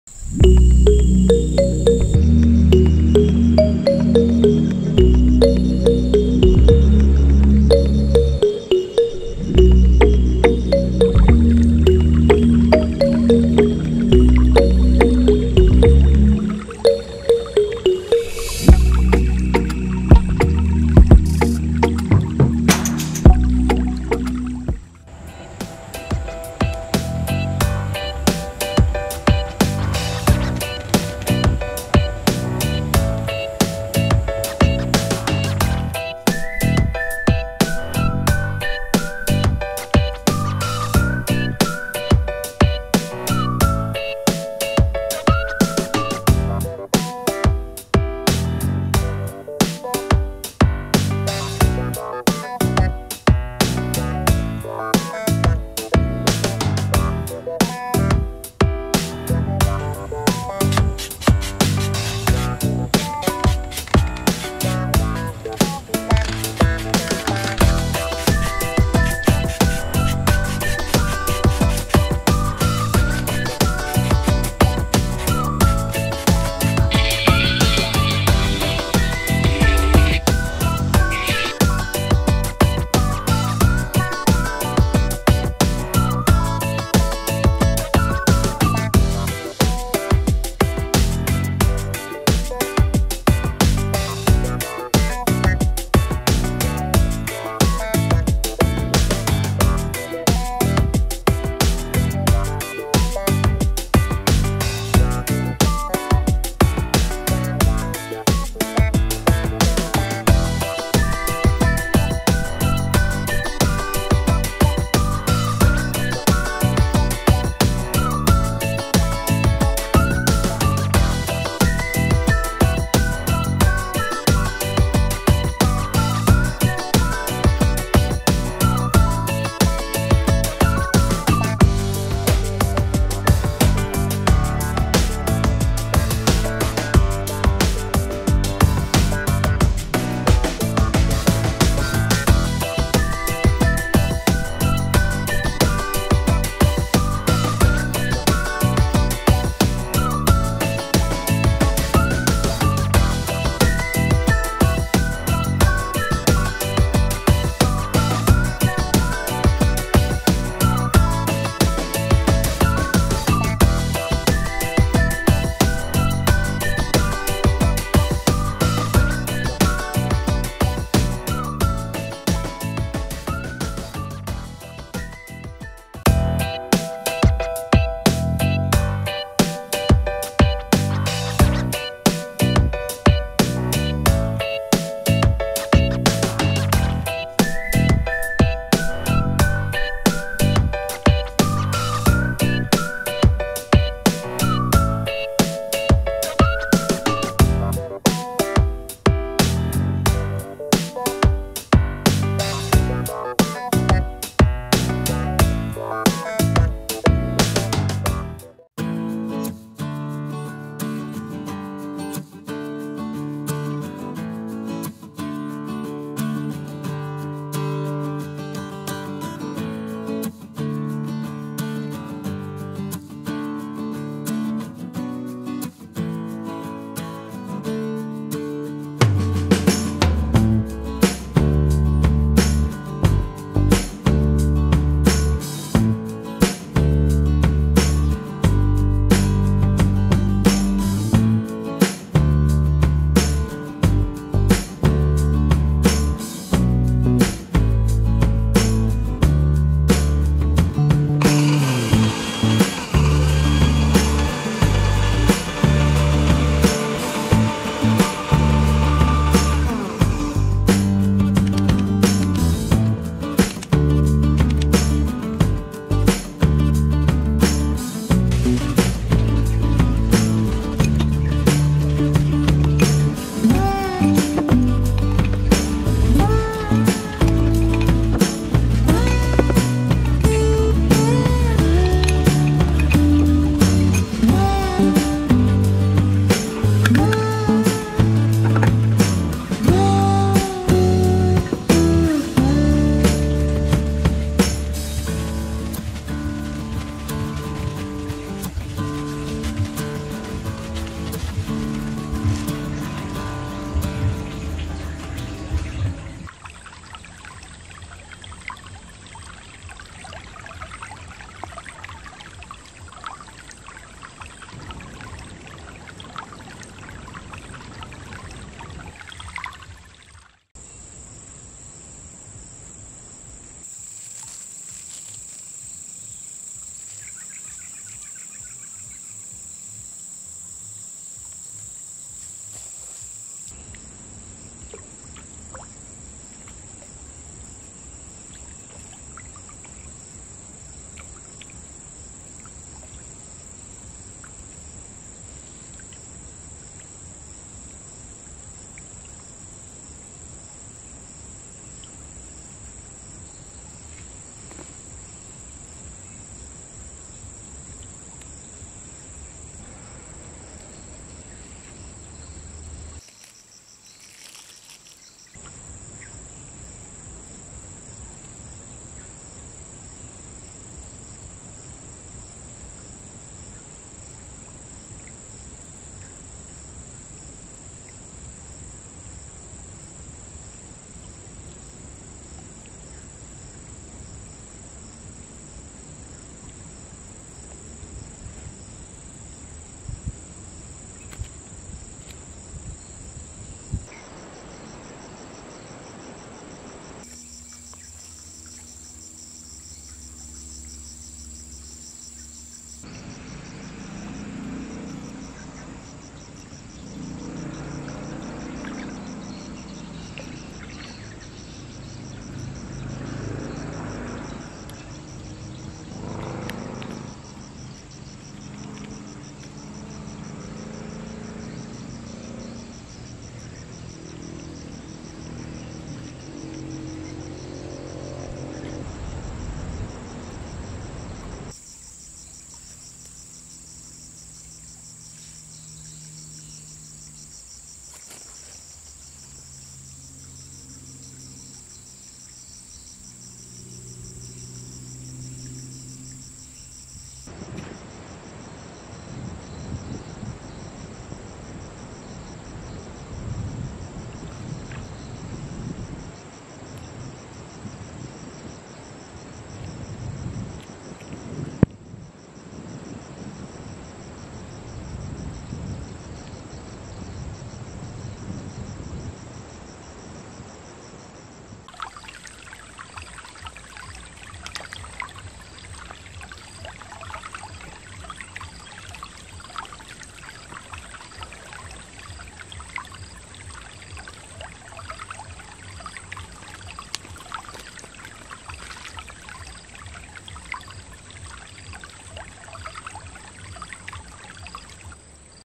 Mancing Wader Membuat Pancing Kecil Dan Umpan Racikan Wader
fiber lentur panjang 110 cm
gagang sikat gigi
tutup botol
spol mesin jahit
fiber
pipa pvc
benang wrapping
ring joran no 4
lem g
lem tembak
amplas

bahan pembuatan umpan

mie instan
bawang putih
masako penyedap rasa
air hangat

piranti pancing

kail no 0,8
senar pancing no 0,19
pelampung kecil
timah lipat

Makes small pliable fishing rods make fishing lures and fishing

110 cm long flexible fiber
toothbrush handle
bottle cap
sewing machine spool
fiber
PVC pipe
wrapping thread
fishing rod no 4
glue g
glue gun
sandpaper

bait making material

instant noodles
garlic
flavored cooking
warm water

fishing tackle

hook no 0.8
fishing line no. 0.19
small buoy
folding tin